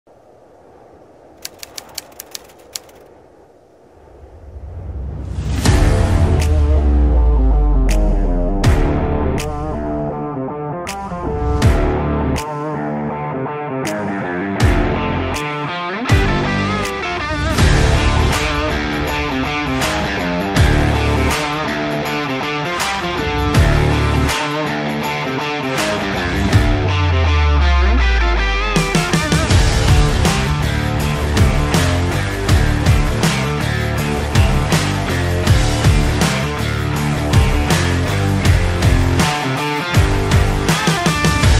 Explore the wilderness | MG HS
When adventure calls, answer it fearlessly with the MG HS. With ADAS safety features, it is equipped to tackle all kinds of terrains and conquer any mountain.

Book now to experience it!